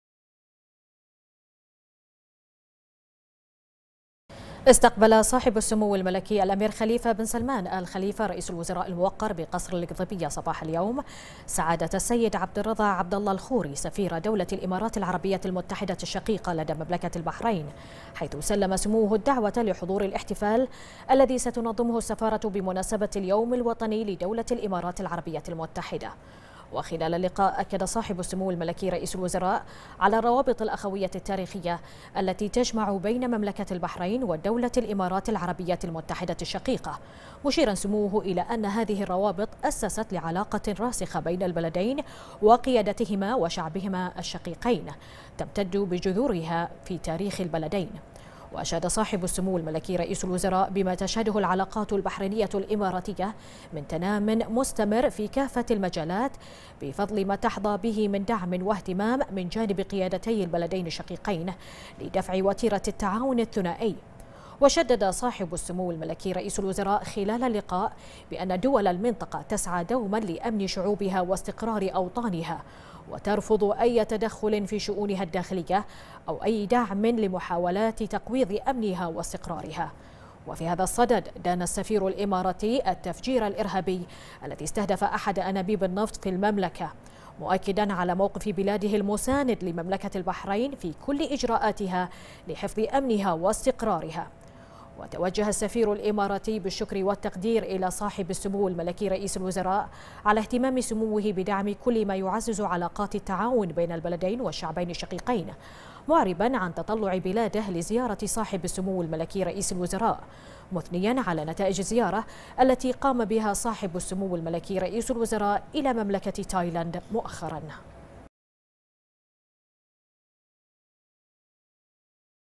سمو رئيس الوزراء يستقبل سفير دولة الإمارات العربية المتحدة لدى مملكة البحرين
المنامة في 15 نوفمبر/ بنا / استقبل صاحب السمو الملكي الأمير خليفة بن سلمان آل خليفة رئيس الوزراء الموقر بقصر القضيبية صباح اليوم، سعادة السيد عبدالرضا عبدالله الخوري سفير دولة الامارات العربية المتحدة لدى مملكة البحرين حيث سلم سموه الدعوة لحضور الاحتفال الذي ستنظمه السفارة بمناسبة اليوم الوطني لدولة الامارات العربية المتحدة.